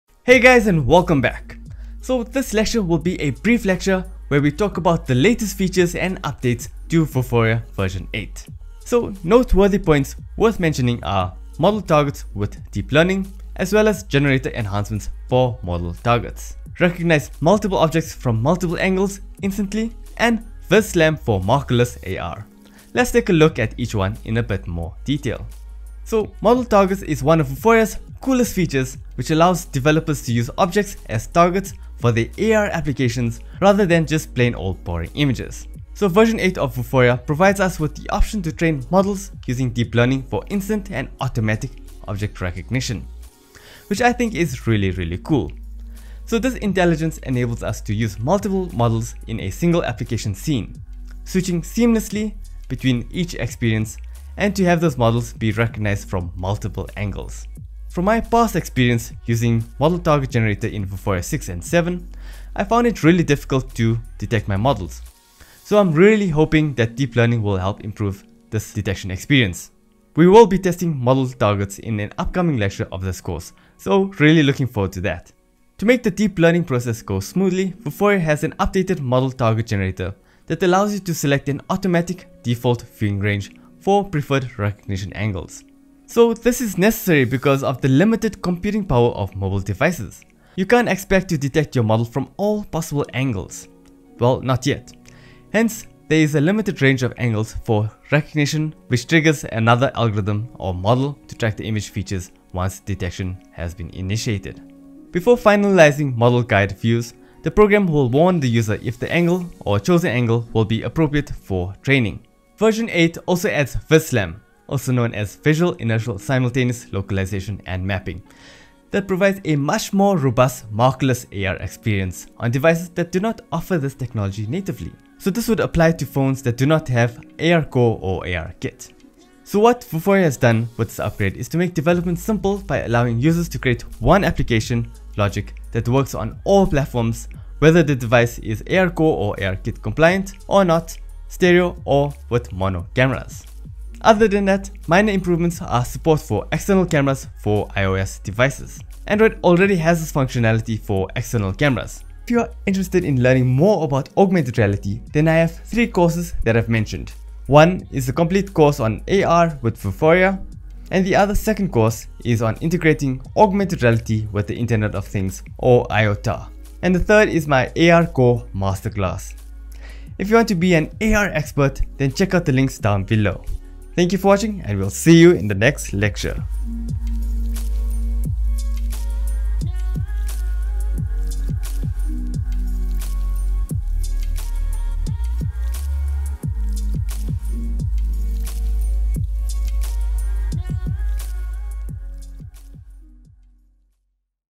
Vuforia 8 Major Updates - Model Targets using Deep Learning, VISLAM and more..
What to learn how to whats new in Vuforia 8? Then check this video on the latest upgrades to Vuforia

Hey guys and welcome back. So this lecture will just be brief lecture, where we talk about the latest features and updates to Vuforia version 8. So the noteworthy points worth mentioning are:

- Model Targets with Deep Learning as well as Generator Enhancements for Model Targets
- Recognize multiple objects, from multiple angles, instantly. and
- VISLAM for markerless AR.

Let’s take a look at each one in a bit more detail.
So Model Targets is one of Vuforia Engine’s coolest features, which allows developers to use objects as targets for their AR applications rather than just plain ol boring images. So version 8 of the Vuforia Engine provides us with the option to train models using Deep Learning for instant and automatic object recognition, which I think I think is really cool. So, this intelligence enables us to use multiple models in a single application scene, switching seamlessly between each experience, and to have those models be recognized from multiple angles.